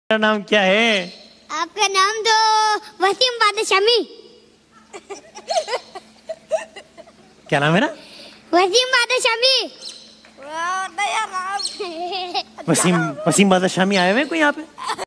Waseem Bada Shami 😊😊☺️😌waseembadami
making of Shan E ramazan transmission stage and effort are countless
it took months to set all this for us.